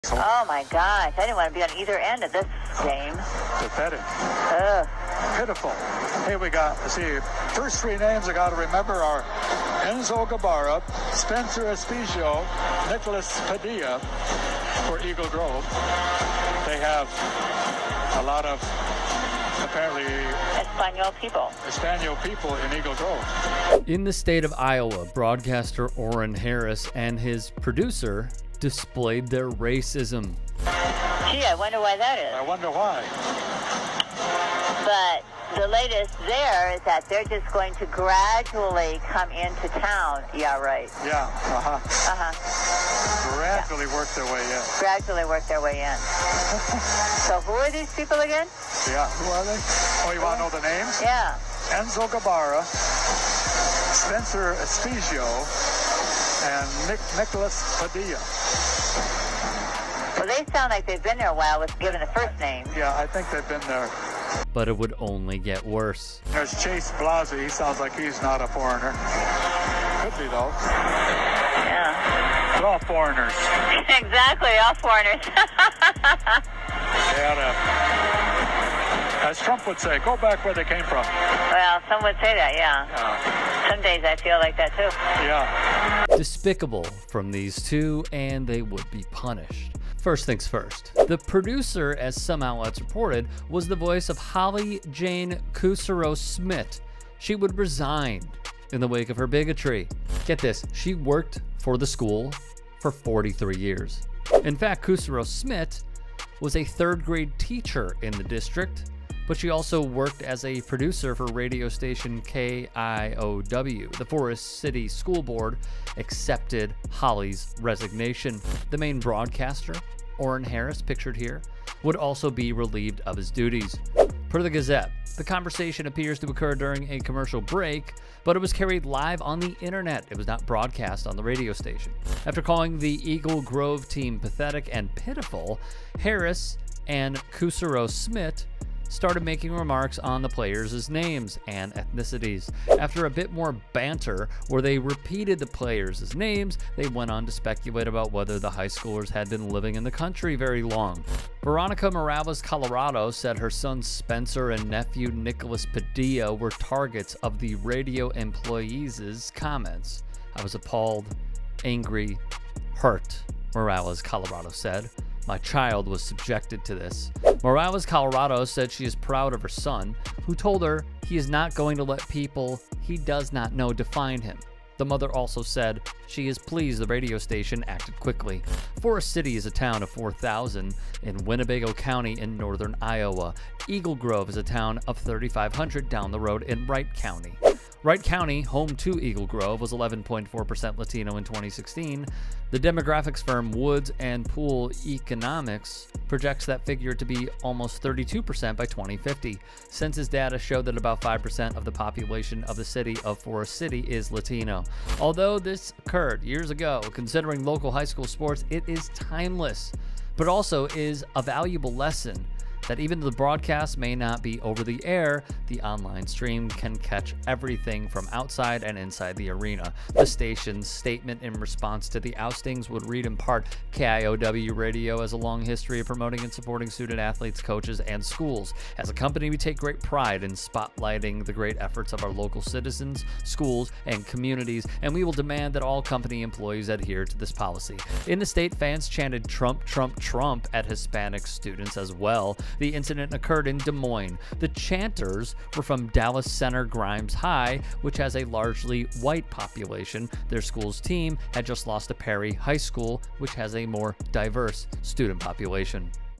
Bigoted Broadcaster Faces The Consequences Of His Actions
A high school basketball broadcaster and his producer have both been relieved of their duties over some racist comments directed towards Latino players. Rick Strom breaks it down.

Rick Strom